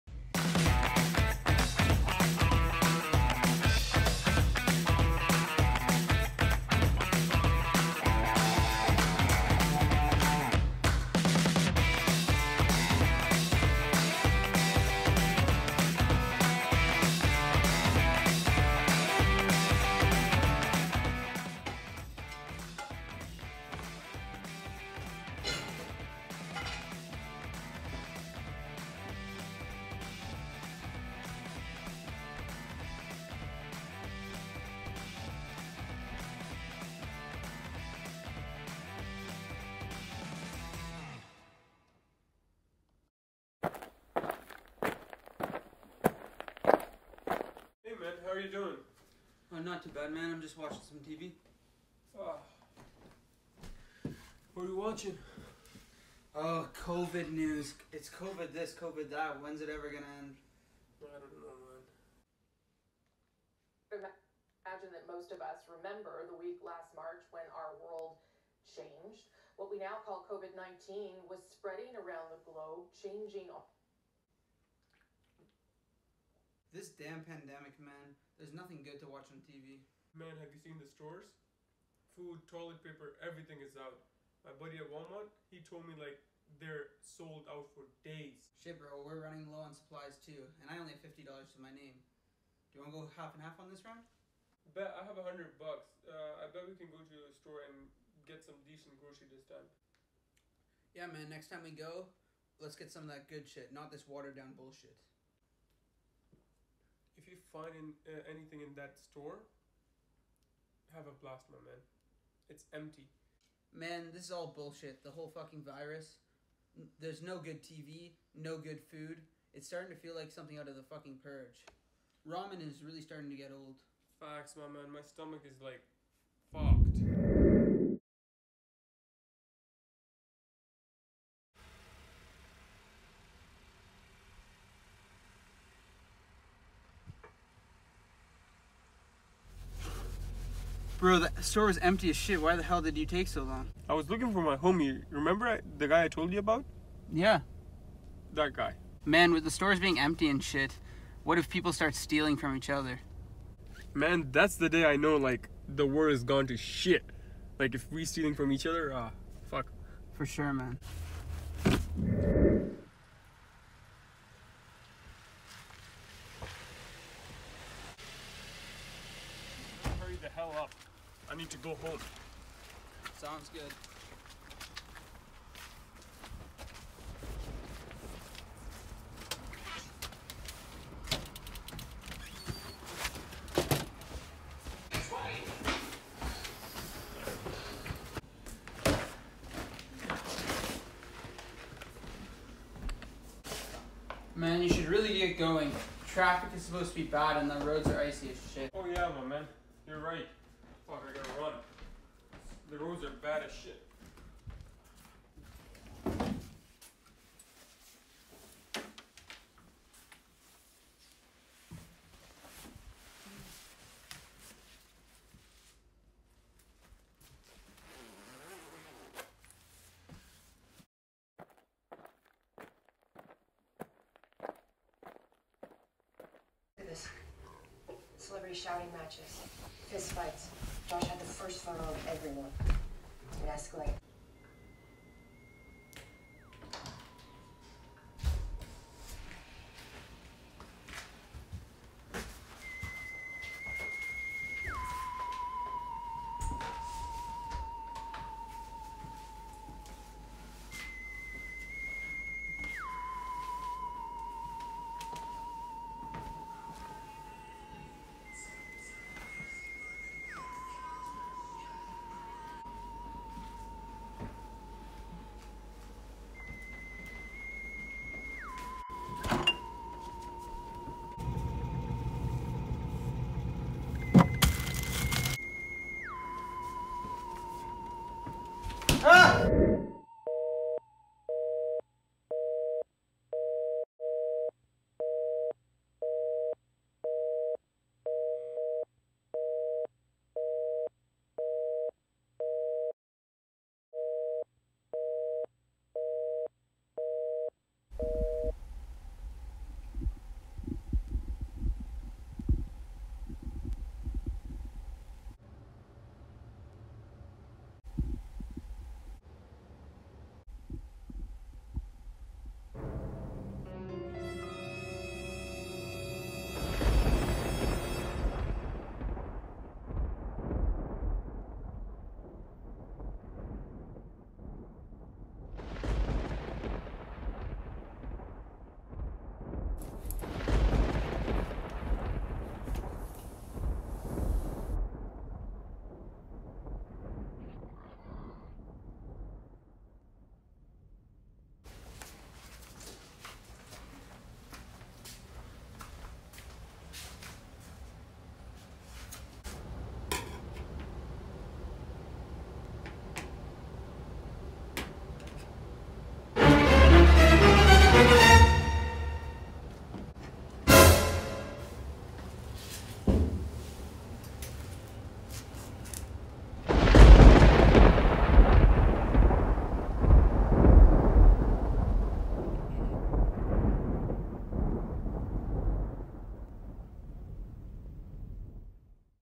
Midnight Heist. 2021 72 Hour Short Film Submission ( Brock University)
Welcome to Midnight Heist, a crime thriller set in fictional world where Covid-19 has been around for 2 years, resulting in a global collapse of the economy and lack of basic commodities in the stores forcing people to result to crime to fend for themselves.  I hope you enjoy